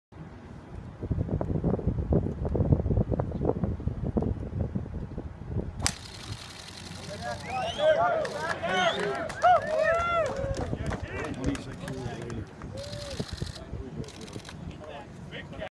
Tiger Woods Tees Off First Tee At Riviera 26 Years After He Began His PGA Tour Career At That Spot
Tiger Woods tees off the first tee at Riviera Country Club in the first round of the 2018 Genesis Open, 26 years after he began his PGA Tour career at exact the same spot (shot from exactly the same camera angle) in the same tournament.